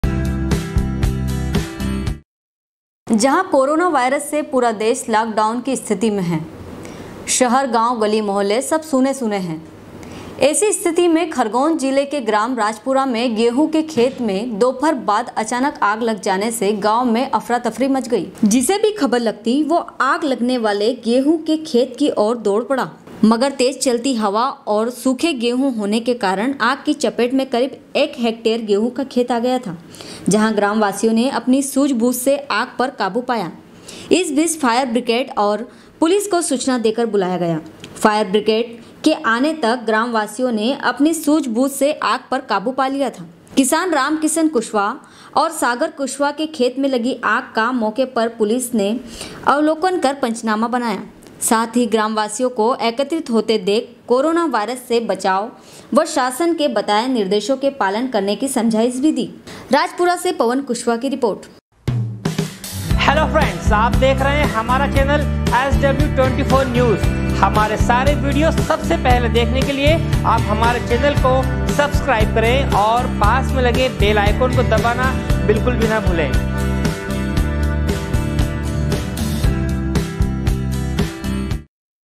sw24news rajpura, 26.03.2020
जहाँ कोरोना वायरस से पुरा देश लाकडाउन की स्थिती में है शहर गांव गली मौहल्ले सब सुने सुने से है, एसी इस्थिती में खरगोन जिले के ग्राम राजपुरा में गेहूँ के खेत में अचानक दोपहर बाद अचानक आग लग जाने से गांव में अफरा तफरि मच गई राजपुर से पवन कुशवाह की रिपोर्ट#sw